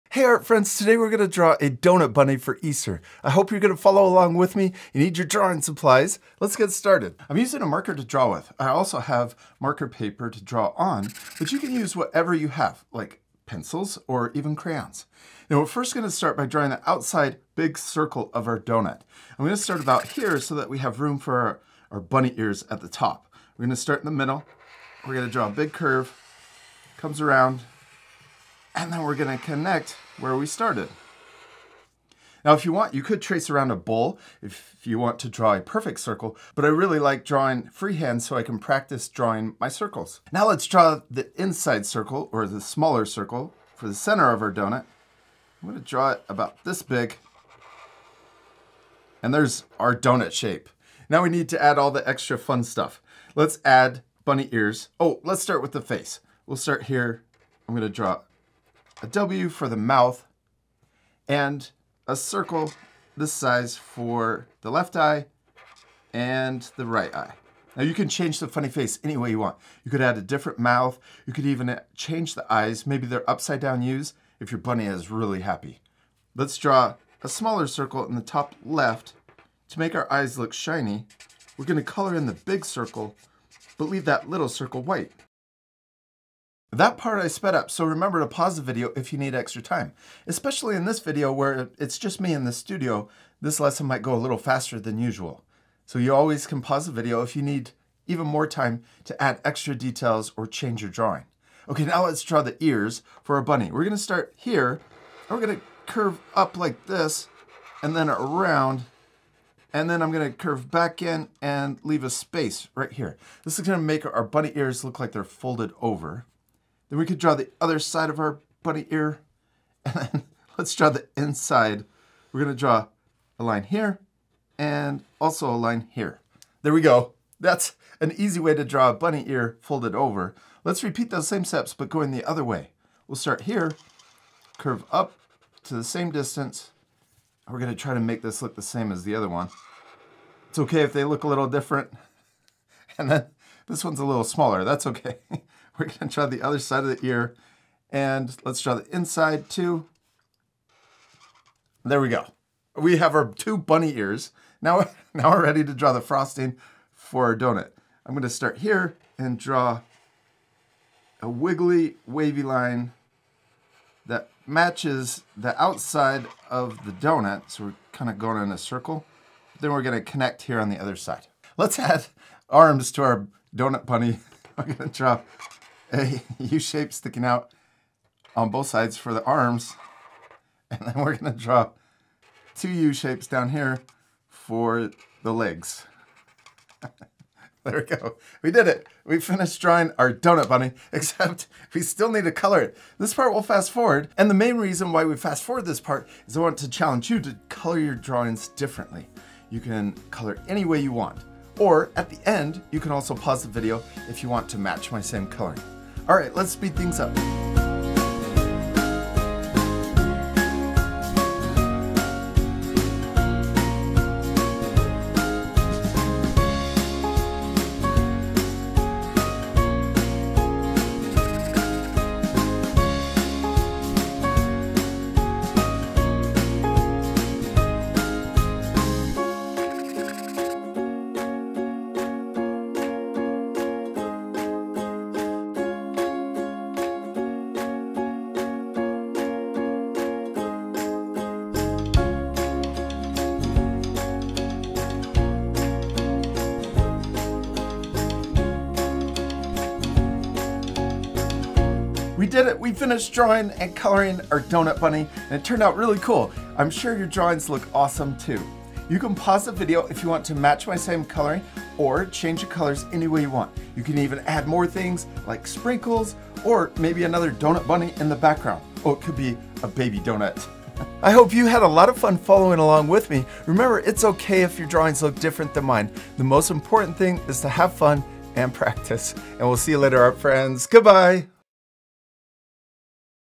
How To Draw A Funny Easter Bunny Donut
Follow along with me and learn how to draw a super funny Easter Bunny donut, perfect for kids of all ages! 🍩🐰

Materials:
Markers 🖍️
Marker paper 📄

Chapters:
00:00 Introduction
00:10 Let's draw a funny Easter Bunny donut
03:11 Time to color!
04:12 Finished!